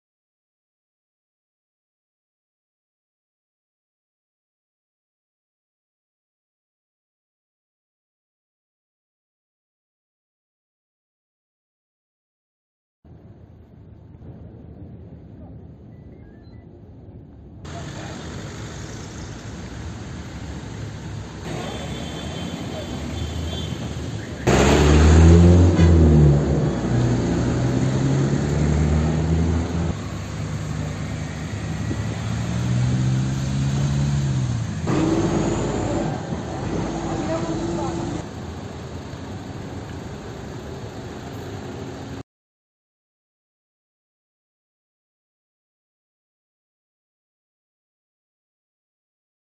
Bursa'daki motorlu gencin feci sonu!
Bursa’da motosikletiyle seyir halinde olan 19 yaşındaki Samet Çobanoğlu, kavşağa geldiği sırada seyir halindeki bir kamyona çarptı. Kamyonun altında kalan genç, ezilerek hayatını kaybetti. O anlar bir işyerinin güvenlik kamerasına yansıdı.